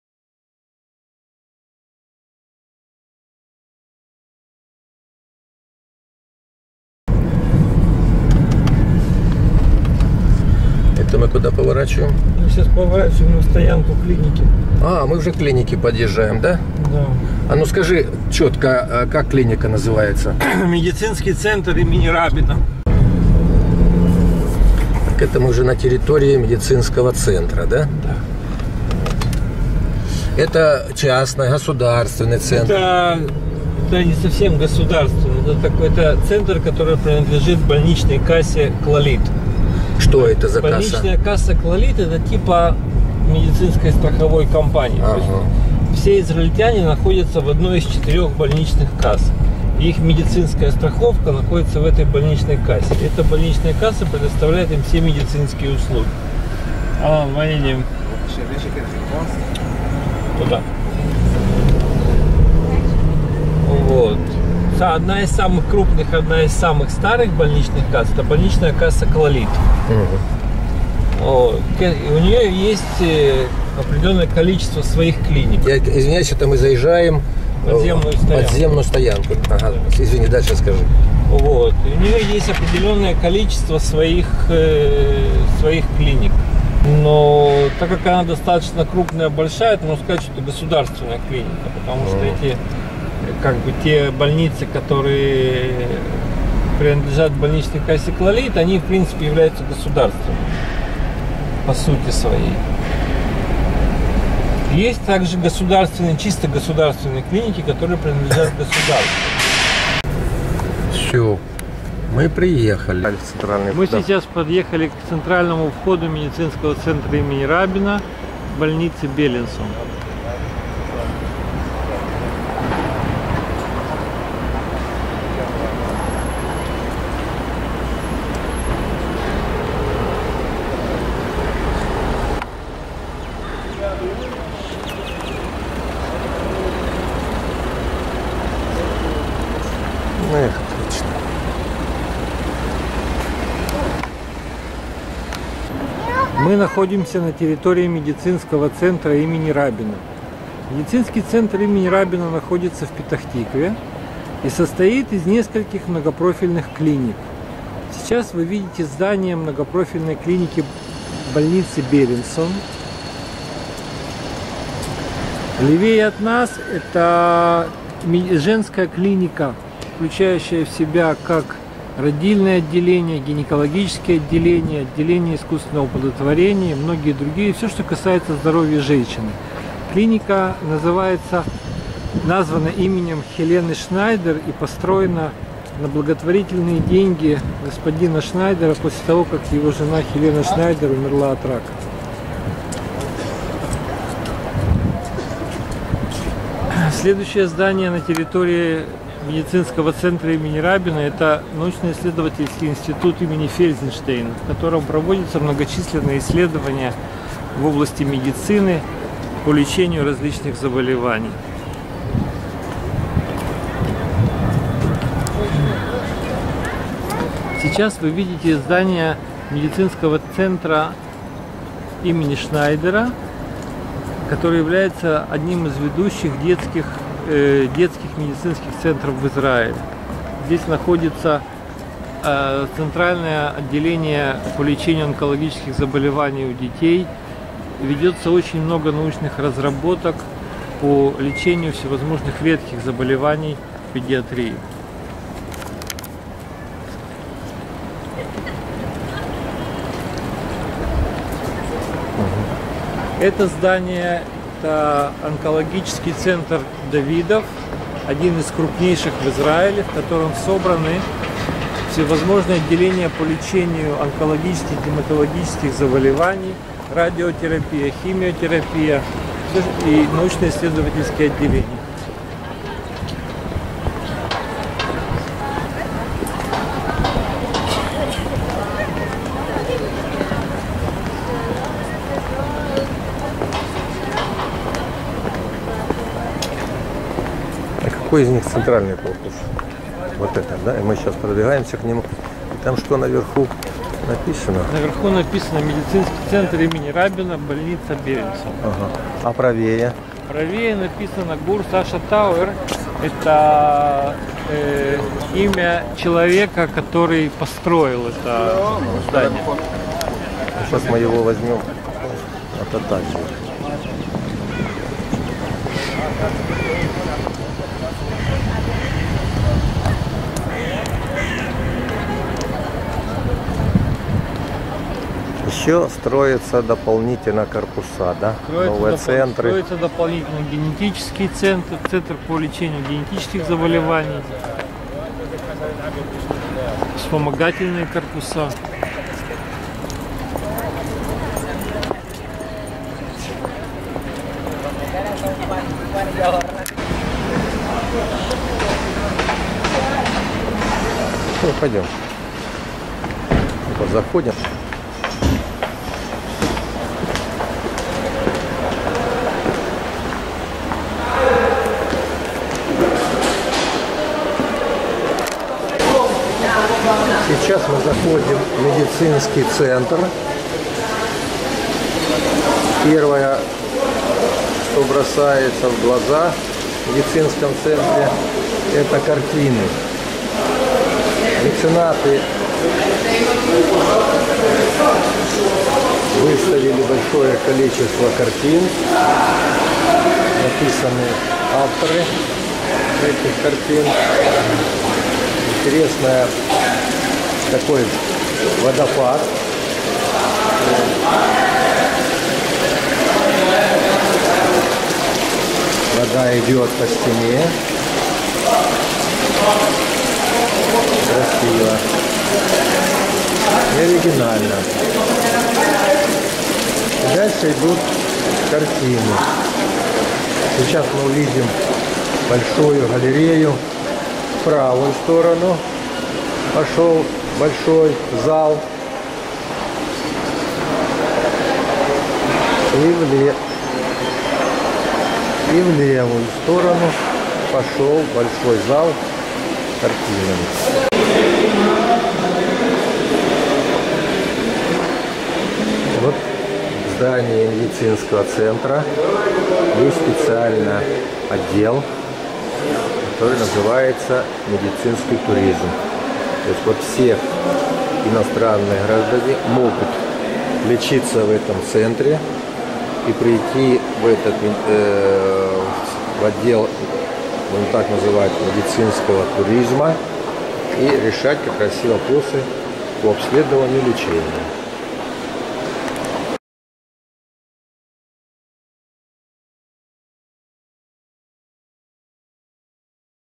Travel to Israel. Part 2. Путешествие в Израиль. Часть 2.
January 2016. Hello to all my friends. I'm in Israel. Today I am in the city of Petach - Tikva (part of a large Tel Aviv). I went for a medical examination. Today, a visit to the medical center Rabin and brief information. In my spare time I will carry out video capture. Yesterday you saw the first part. Today, the second part of my report about the journey to Israel.
Январь 2016 года. Привет всем моим друзьям. Я в Израиле. Сегодня я в городе  Петах - Тикве  (входит в большой Тель Авив). Я приехал для медицинского обследования. Сегодня посещение медицинского центра имени Рабина и краткая информация.  В свободное время буду проводить съемки видео. Вчера Вы видели первую часть. Сегодня вторая часть часть моего репортажа о путешествии в Израиль.